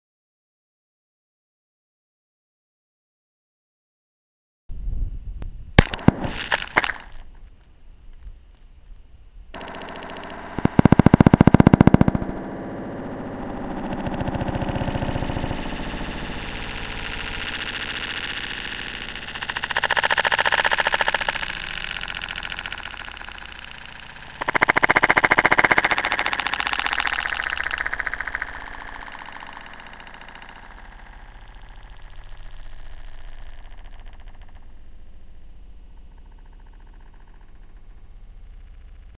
25 06
Stuff guys do.  A few minutes at the Ajo Rifle Range with a 25-06, a half dozen plastic water bottles and a GoPro camera.  Fired a single shot into the line of bottles set at the 200 yard line with a 117 grain bullet.  The round bit earth after passing through the third bottle as documented here.  Pretty dynamic energy.